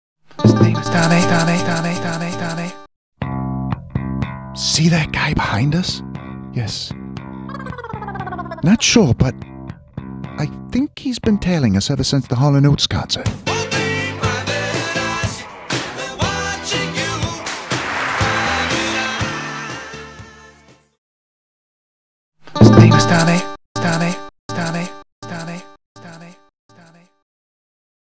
Tom and Carol and a Creeper
In 30 second's time, Tom gives Carol a warning about an anonymous creeper. Minimal stop-motion animation. Jeffrey Thomas Cook creation/voices.